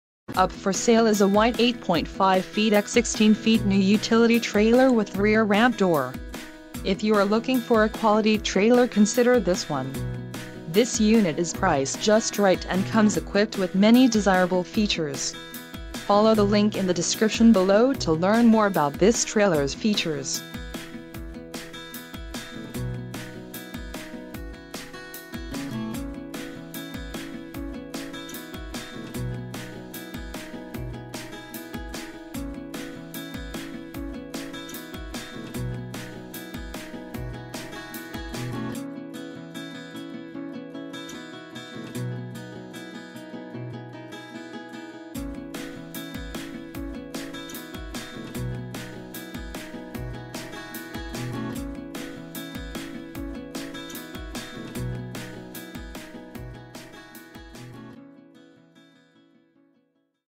White 8.5' x 16' New Utility Trailer w/ Rear Ramp Door -...
This Utility Trailer is ready for immediate pickup from our Tampa lot.  The white metal exterior looks especially sharp when being pulled by the same color vehicle.  The RV style side door with flush lock looks very sleek.READY on the Tampa (FL) lot NOW!! LOW Tampa LOT PRICE is ONLY $3887Factory retail price of this White trailer is $3599 (factory in Georgia.This trailer may be eligible for financing.